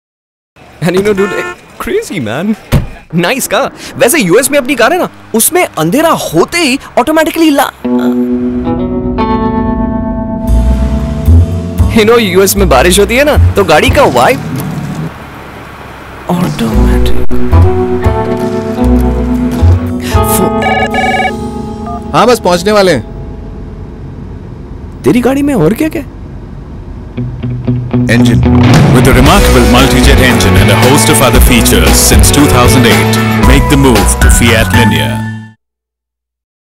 Fiat US return
This Beautiful and effective Ad film of Fiat US return Directed by Nitish Sharma.